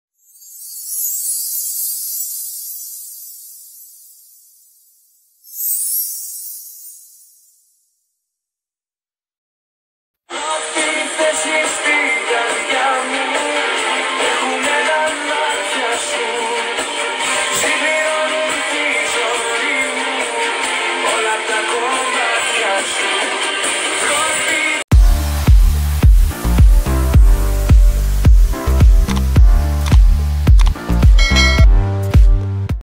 Αποκλειστικό: Δείτε τι έκανε το αγόρι της Καινούργιου Φίλιππος ενώ η Κατερίνα έκανε πρεμιέρα !!!!
Τελικά δεν ήταν όλα λάθος Κατερίνα ήταν εκεί το αγόρι σου!!! να σε καμαρώνει